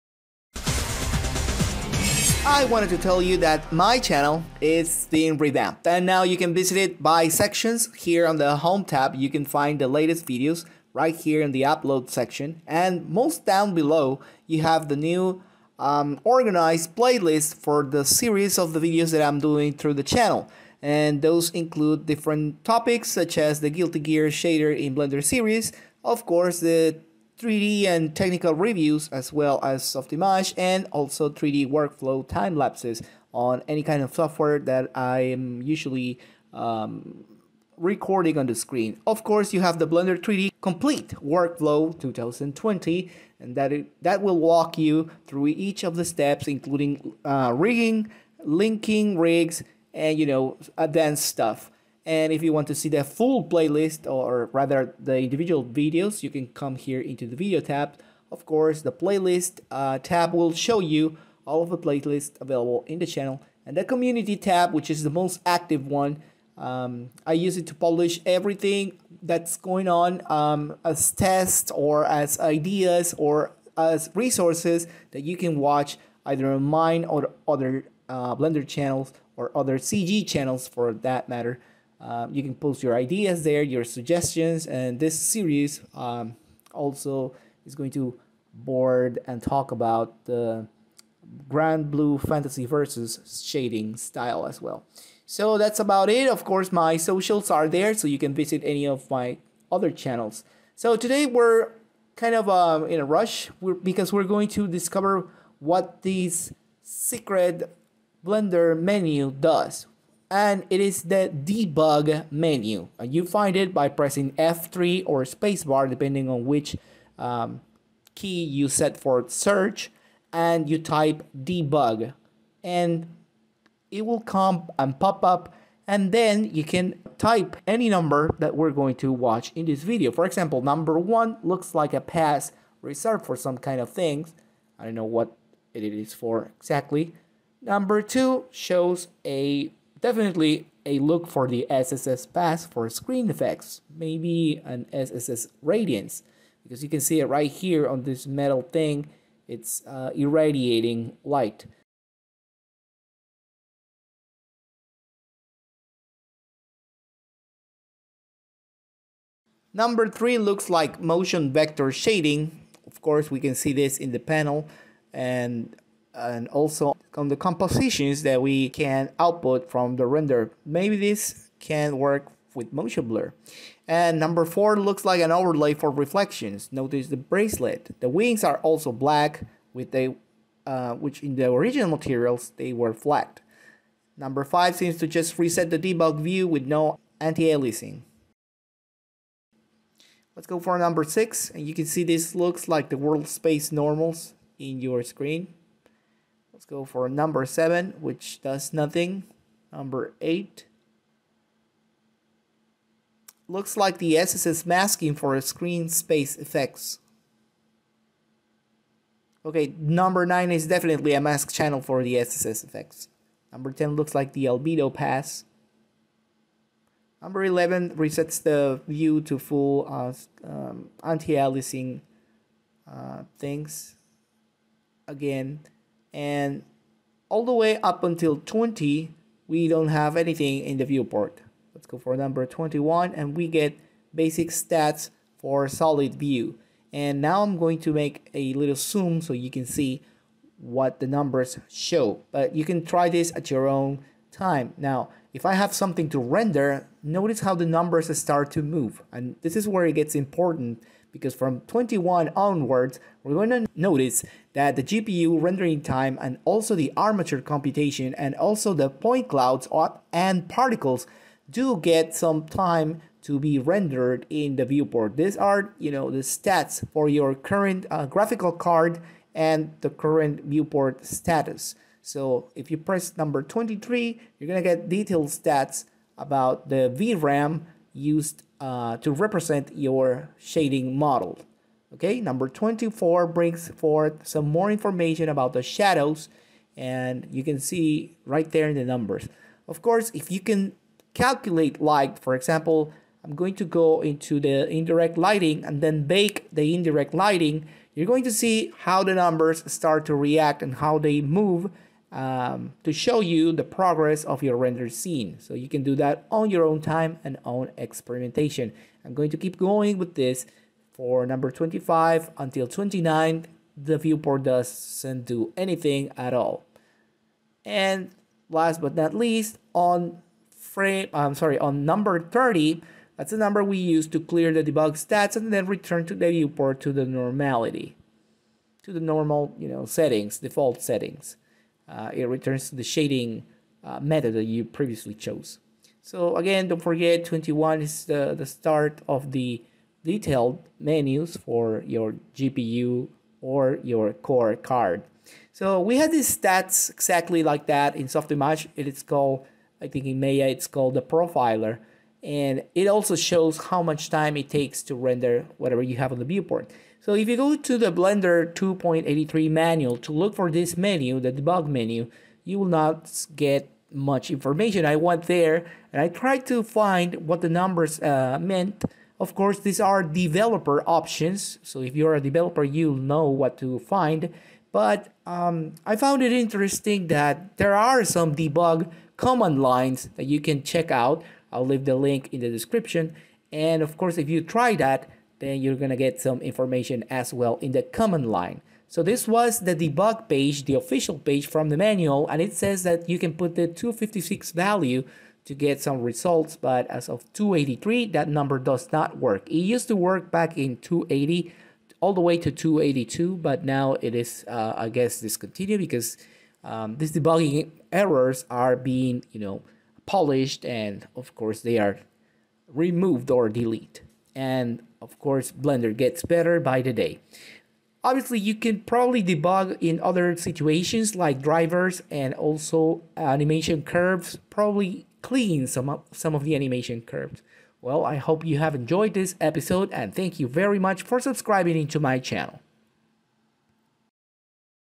Eevee secret menu (Blender 2.83)
Blender has a debug status for the viewport, so you can optimize and track all the effects, shader compilation and render for your work.
This is known as the "profiler" in other applications.

Since I asked around the forums and apparently no one knows how to pilot the DEBUG codes in the viewport, I decided to study the Softimage Scene stats and translate some of the terms of use. I guess they are similar as well. Thanks!